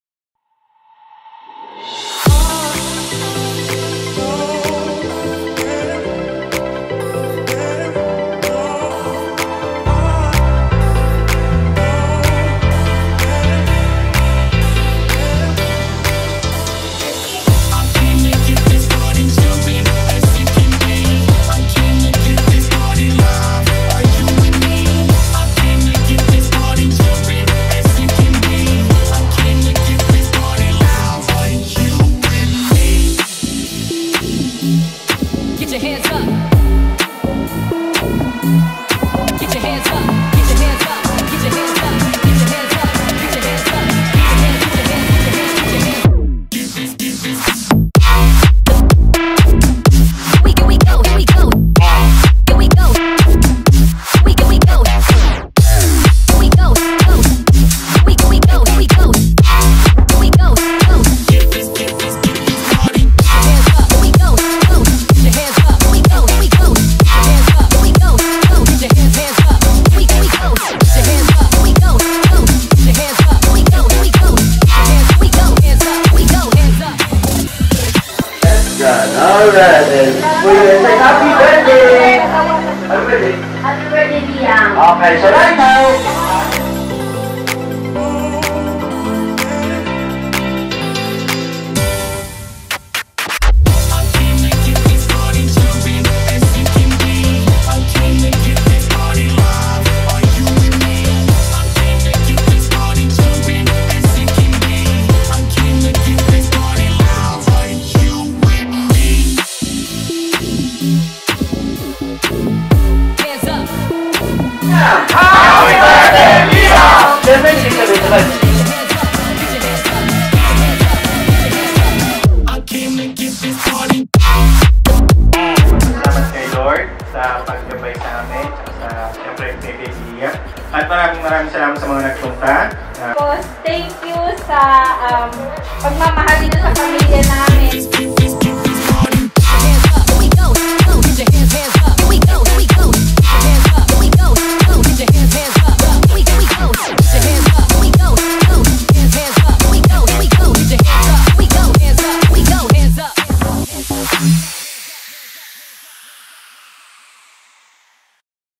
liam's 1st b day
celebration of Liam's first birthday held in icook restaurant